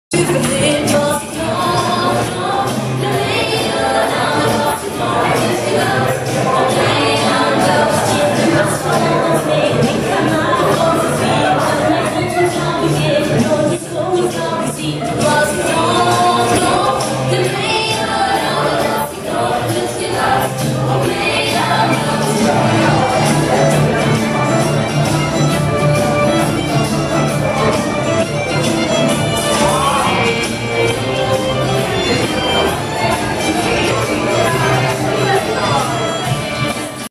Andrzejki 2016: uczniowie G17 w Rybniku tańczą Belgijkę
W piątek 25 listopada 2016 roku w auli szkolnej ZS nr 1 im. Powstańców Śląskich w Rybniku gimnazjaliści bawili się na imprezie andrzejkowej, w ramach której odbyły się otrzęsiny klas pierwszych. Nie zabrakło wspólnych tańców, czego dowodem jest Belgijka :)

Zobaczcie, jak bawili się uczniowie Gimnazjum Dwujęzycznego nr 17 w Rybniku!